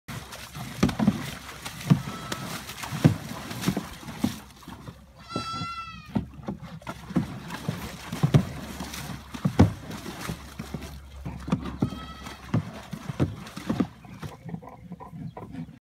Magic the horse splashing in his water bucket
It was a hot day outside. Magic decided to cool off by playing on his water trough.